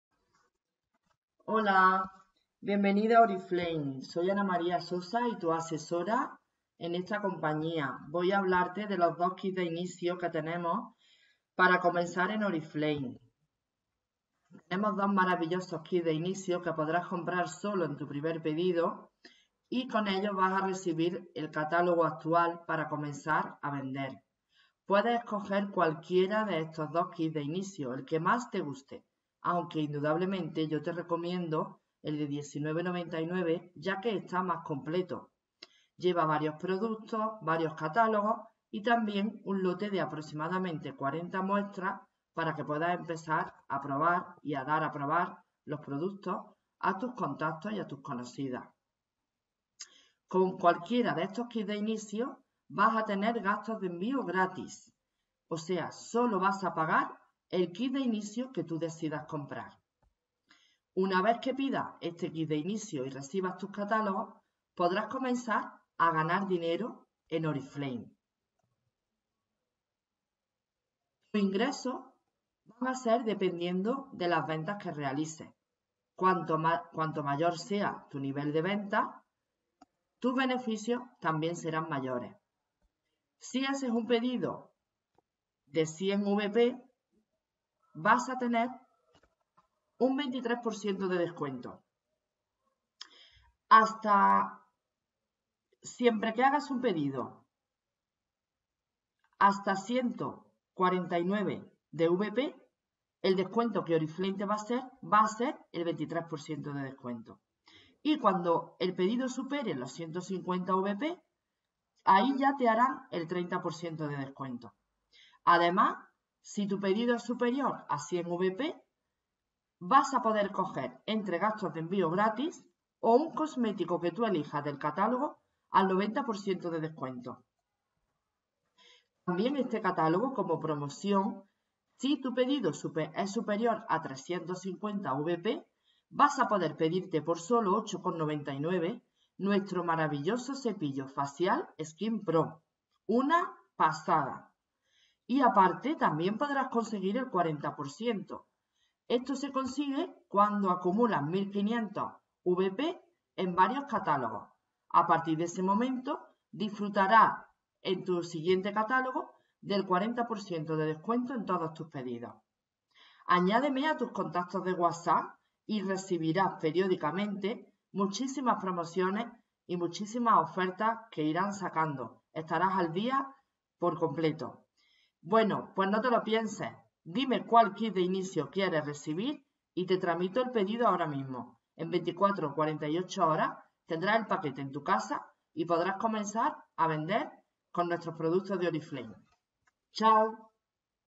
Kit de inicio catalogo 9 de Oriflame
hazte distribuidora de Oriflame y gana un dinero extra, en este vídeo te explico como puedes empezar en Oriflame pidiendo el kit de inicio